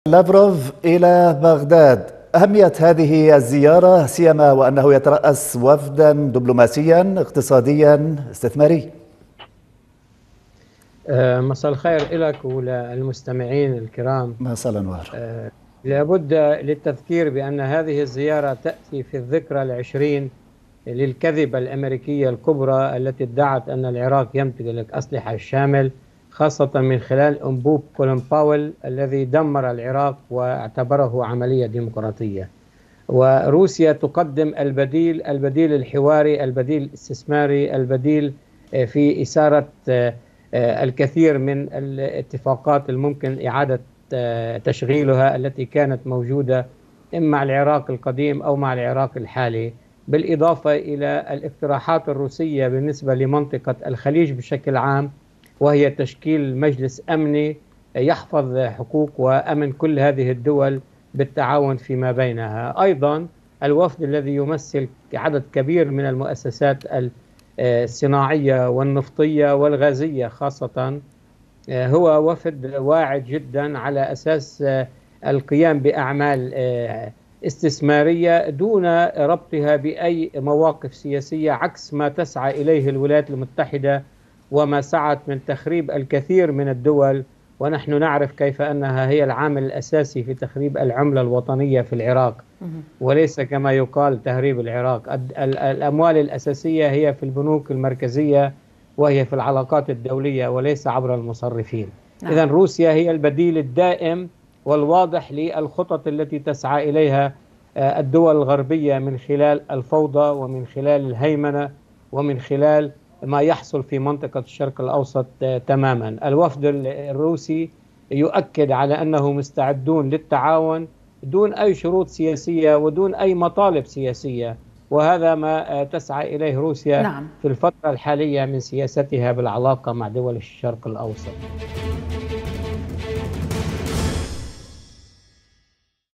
ما سبب زيارة وزير الخارجية الروسي لـ بغداد ؟
وزير الخارجية الروسي يصل بغداد على رأس وفد رفيع في زيارة رسمية

موقع قناة التغيير الفضائية  الالكتروني